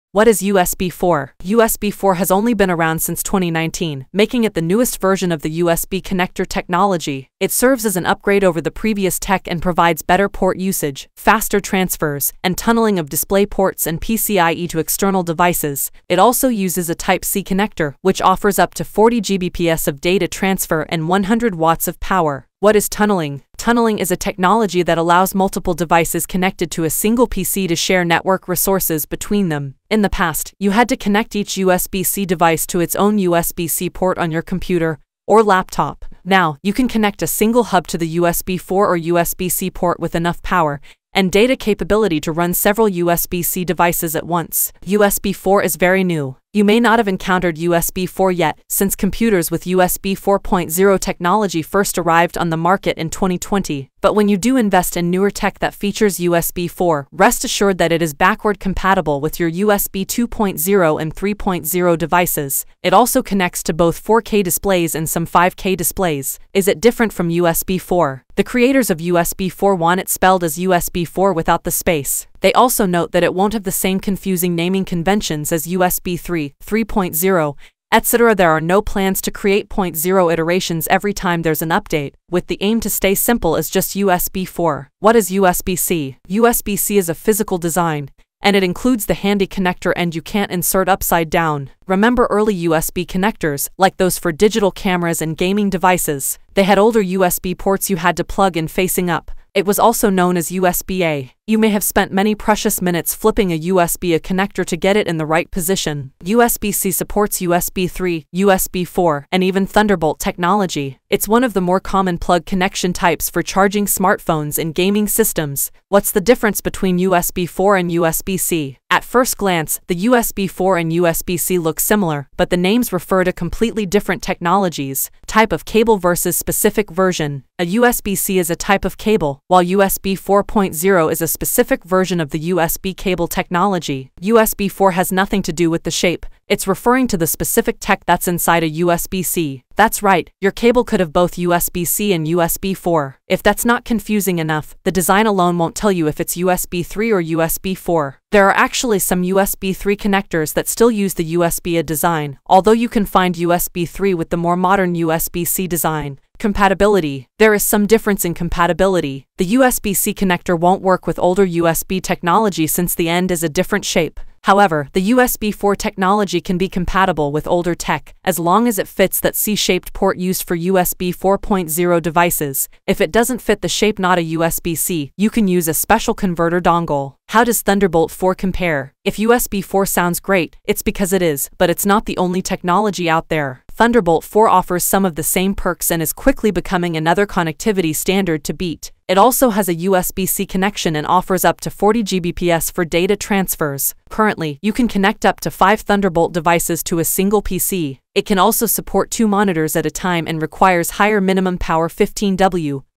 What is USB4,USB-C and Thunderbolt 4?_[RAY TRACE]
What is USB-C

USB-C is a physical design, and it includes the handy connector end you can’t insert “upside down.” Remember early USB connectors, like those for digital cameras and gaming devices? They had older USB ports you had to plug in facing up. It was also known as USB-A. You may have spent many precious minutes flipping a USB-A connector to get it in the right position.

What is USB 4?

USB 4 has only been around since 2019, making it the newest version of the USB connector technology. It serves as an upgrade over the previous tech and provides better port usage, faster transfers, and "tunneling" of display ports and PCle to external devices. It also uses a Type-C connector, which offers up to 40 Gbps of data transfer and 100 watts of power.

How does Thunderbolt 4 compare?

If USB 4 sounds great, it’s because it is, but it’s not the only technology out there. Thunderbolt 4 offers some of the same perks and is quickly becoming another connectivity standard to beat. It also has a USB-C connection and offers up to 40 Gbps for data transfers.
Currently, you can connect up to 5 Thunderbolt devices to a single PC. It can also support two monitors at a time and requires higher minimum power (15W for Thunderbolt vs. 7.5 for USB4.)

Thanks